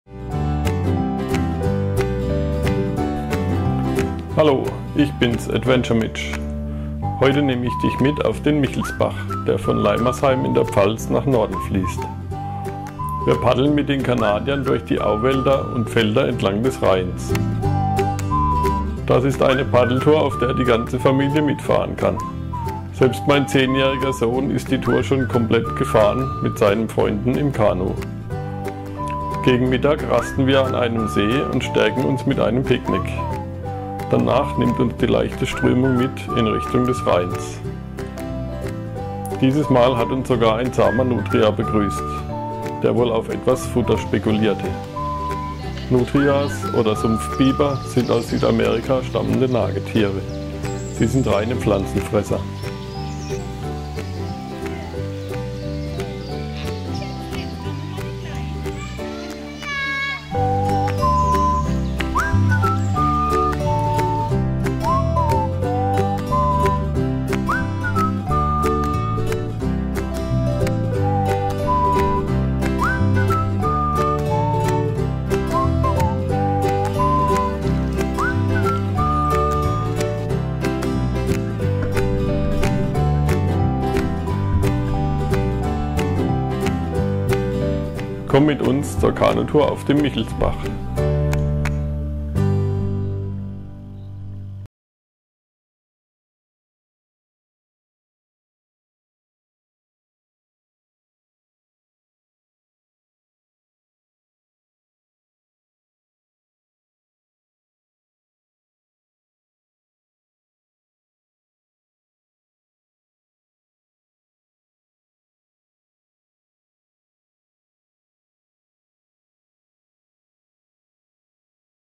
Kanu fahren Michelsbach, Leimersheim, Pfalz, Altrhein. Paddeln im Kanadier
komme mit AdventureMitch zum Kanufahren auf den Michelsbach in der Pfalz. Der Michelsbach fließt in einem großen Bogen von Leimersheim nach Norden. Zwischen Feldern und durch den Wald führt uns die Strömung auch mal an einem Angelweiher vorbei bis in den Sondernheimer Altrhein.